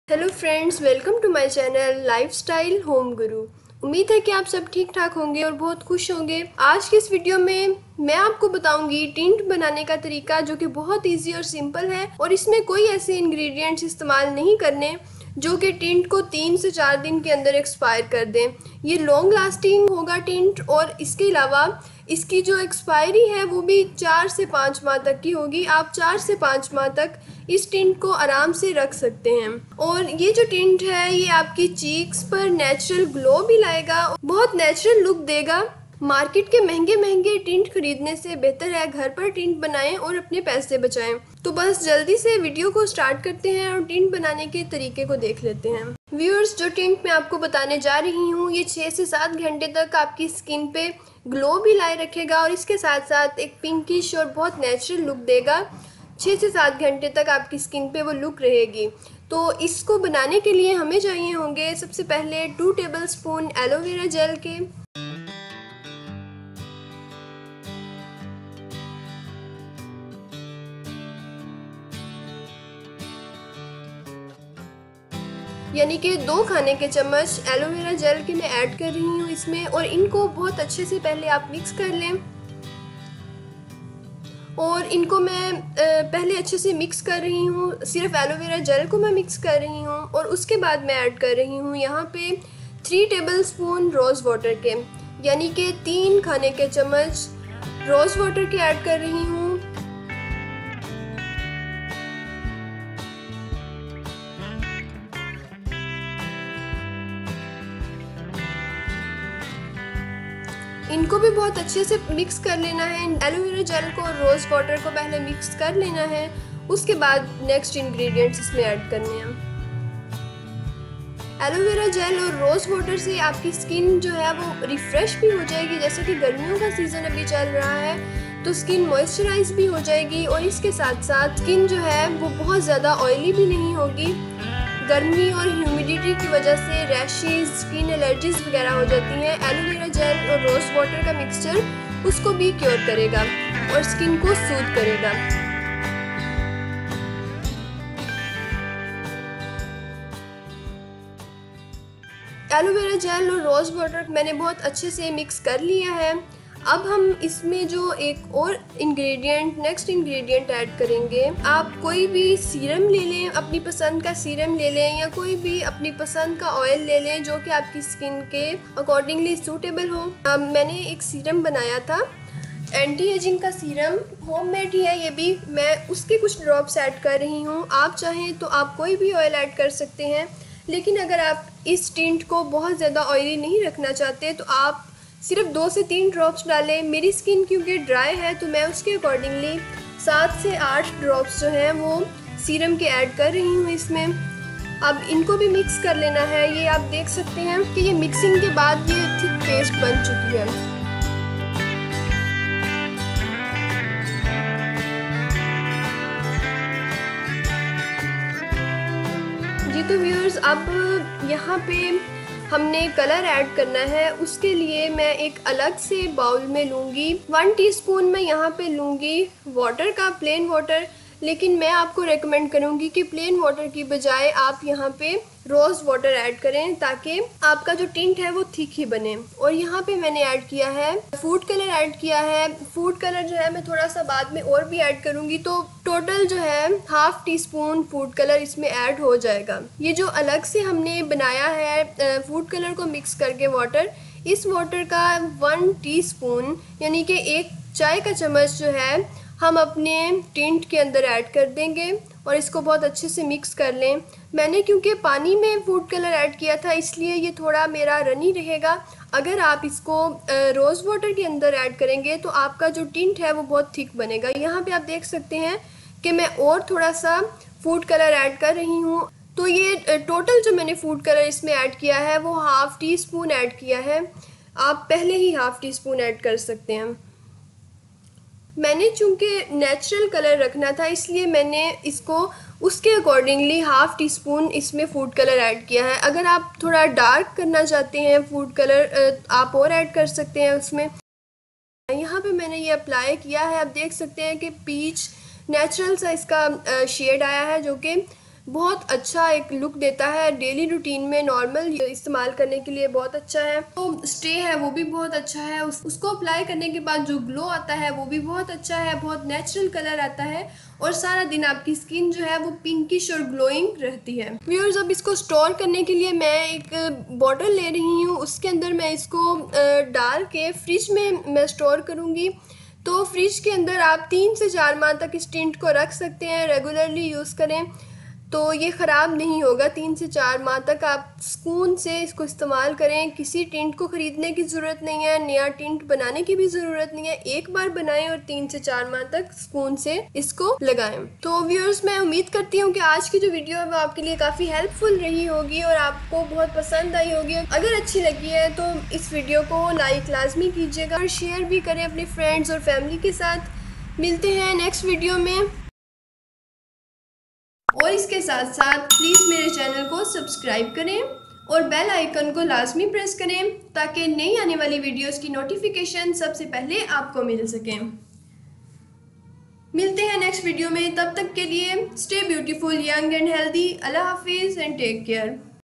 Lip and cheeks tint - Natural tint - Homemade Tint - Long-lasting Tint for lips and cheeks -DIY
We made homemade tint for lip and cheeks by using naturalingredients & foodcolour. I have tried many homemade tints but this experiment is just WOW. This tint having beautiful pigment and gives long-lasting pinkish glowing skin. I request you all to must try it out and give your feedback in the comments section

We Look forward to Serving you the Very Best!